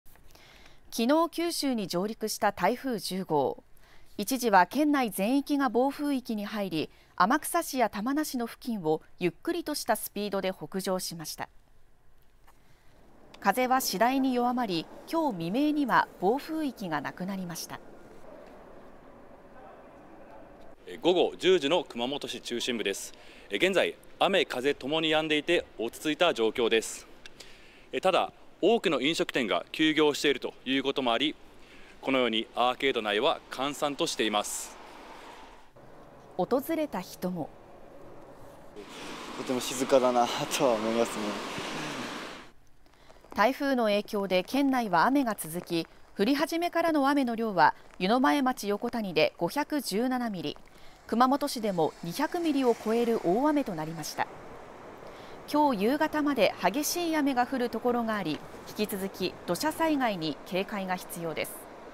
【台風10号】熊本県 暴風域なくなるも夕方まで激しい雨のおそれ 引き続き土砂災害に警戒（8月30日(金)正午時点）
台風10号はきょう30日(金)未明に熊本県内を抜けましたが、午後も激しい雨が降る所がありそうです。